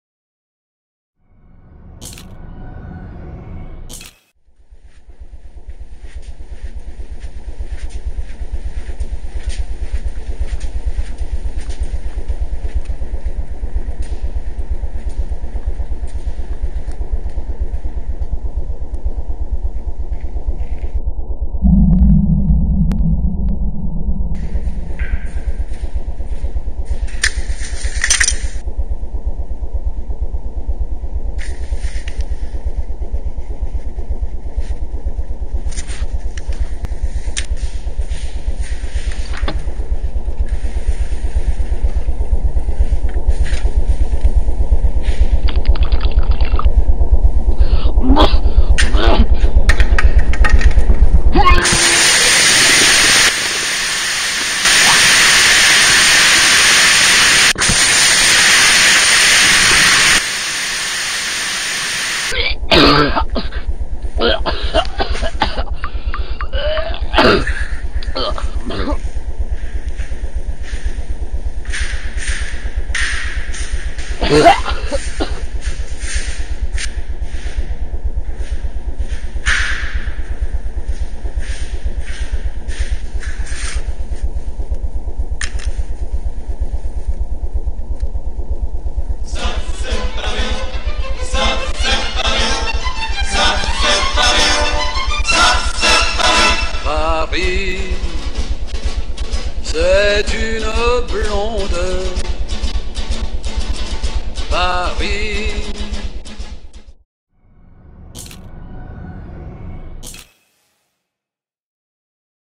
JVC Adixxion - Lorenzesko Fondaruk: Chemicznego
Abbiamo fatto testare Adixxion, l'action camera di JVC al regista Lorenzo Fonda che ha realizzato in poche ore un corto e ne ha approfittato per raccontarci un po' di se, del suo lavoro e del nuovo documentario che sta realizzando.